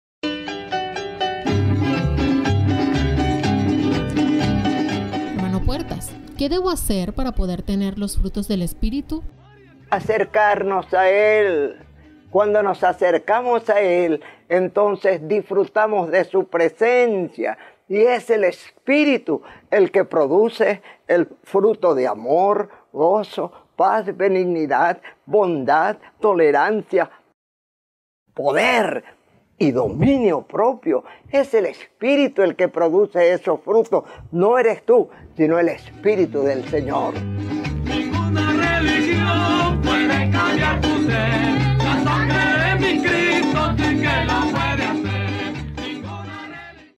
HNO PUERTAS RESPONDE #046 | ¿LOS FRUTOS DEL ESPÍRITU? | MICRO HNO PUERTAS | LUZ TV
pastor Jaime Puertas:
 ¿QUÉ DEBO HACER PARA PODER TENER LOS FRUTOS DEL ESPÍRITU?